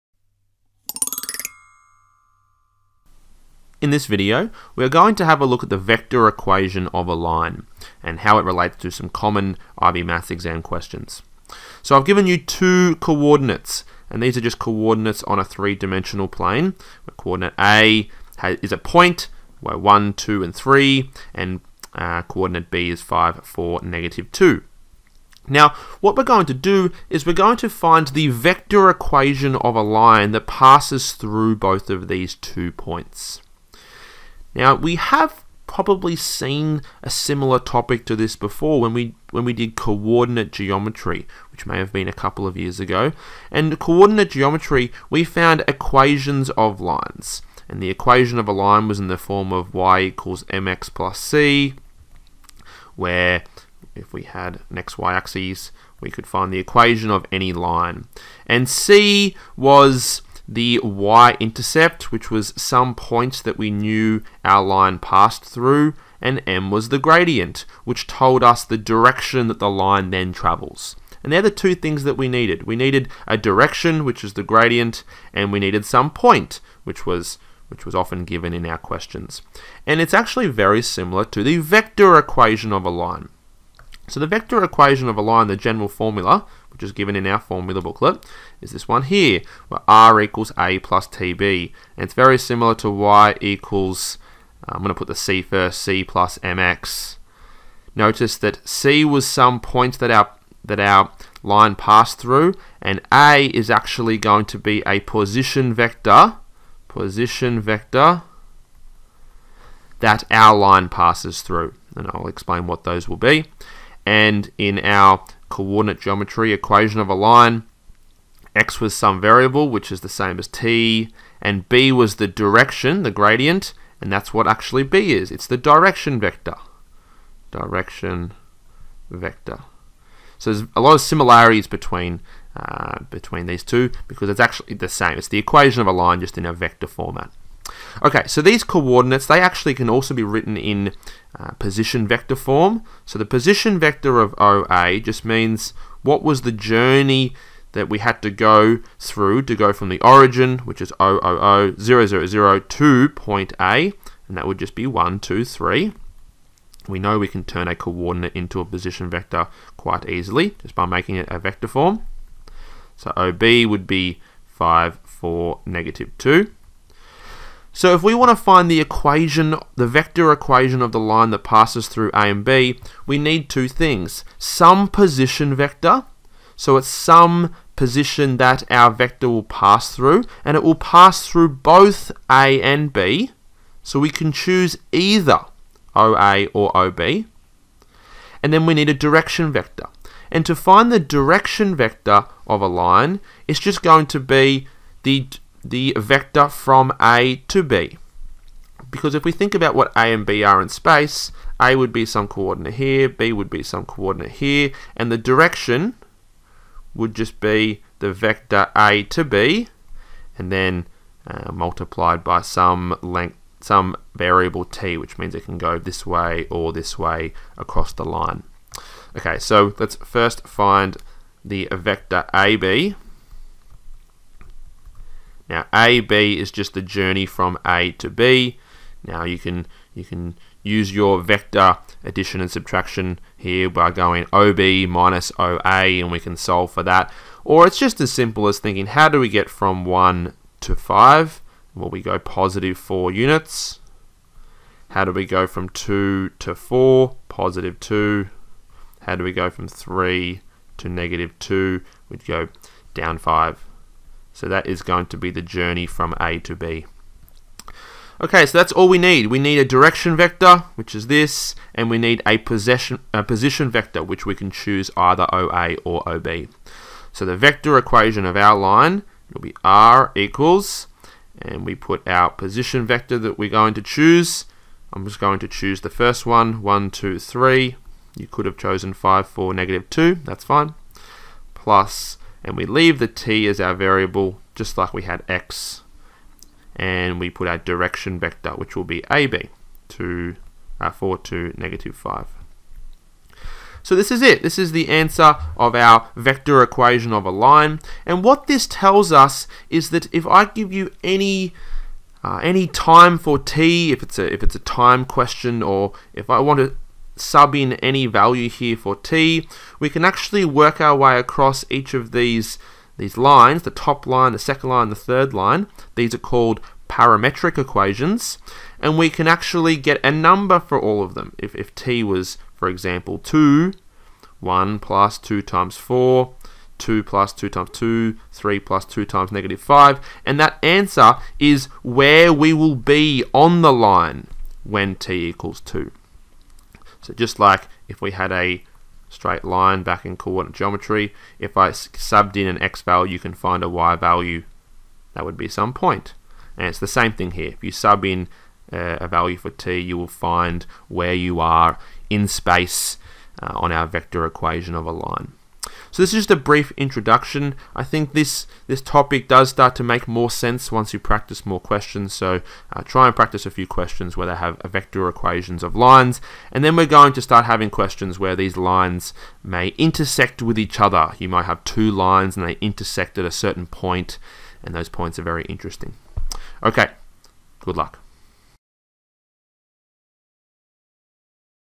Vector Equation of a Line [IB Math AI HL]
This video covers the Vector Equation of a Line. Part of the IB Mathematics Applications & Interpretation HL course in Topic 3, Geometry & Trigonometry.